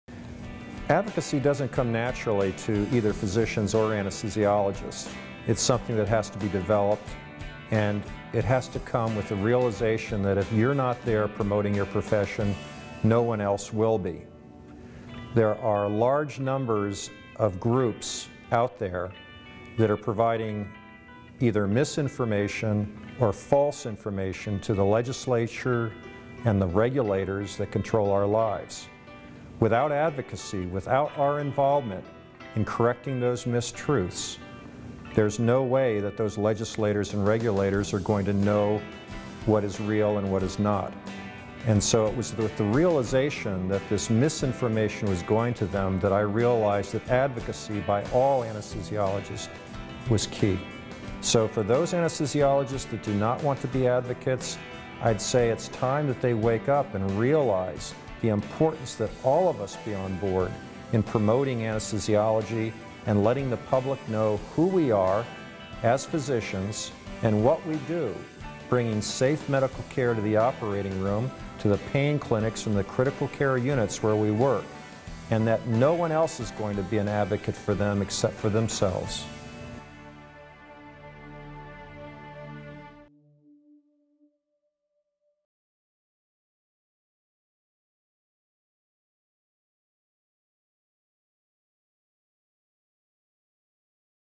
Anesthesiologists need to get involved to make a difference
ASA Past President Roger Moore, M.D., explains the importance of anesthesiologists getting involved in advocacy to positively influence the future of the profession.